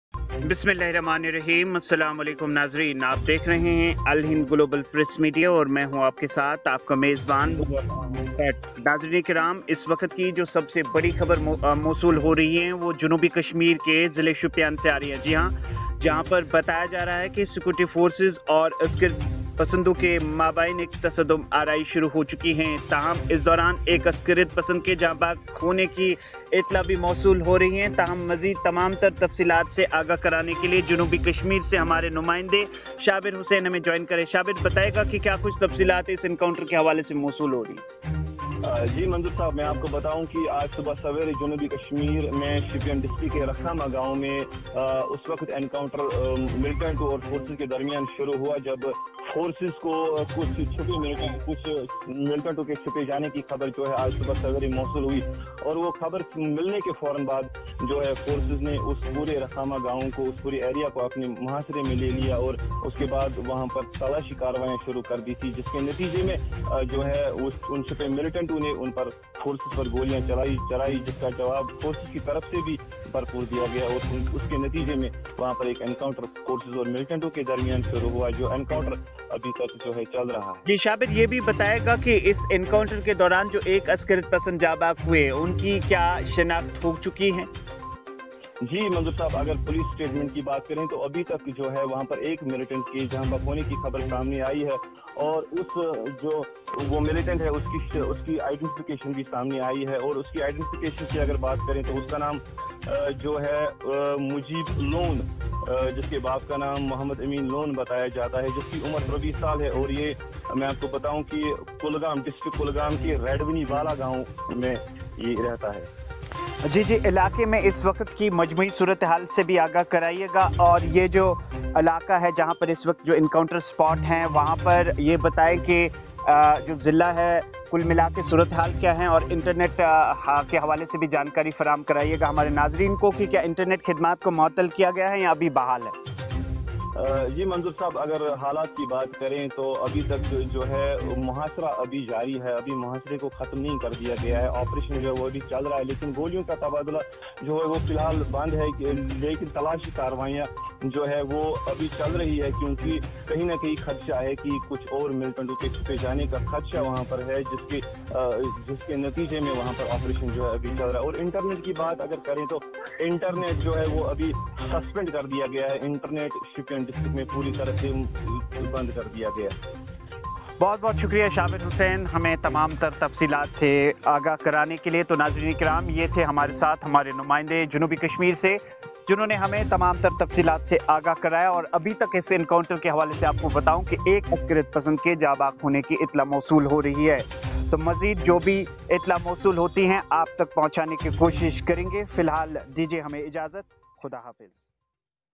BreakingNews SHOPIAN ENCOUNTER: 01 MILITANT KILLED, OPERATION ON
SHOPIAN ENCOUNTER: 01 MILITANT KILLED, OPERATION ON

Srinagar, October 01:One militant has been killed in an ongoing encounter at Rakhama area of South Kashmir’s Shopian district on Friday.

Official sources told GNS, that one militant has been killed in an ongoing encounter, however identity of the slain militant is yet to be ascertained.
Earlier Reports reaching GNS that a joint team of Police and army launched a cordon and search operation in Rakhama.

As the joint team of forces approached towards the suspected spot, the hiding militants fired upon the forces, triggering off an gunfight.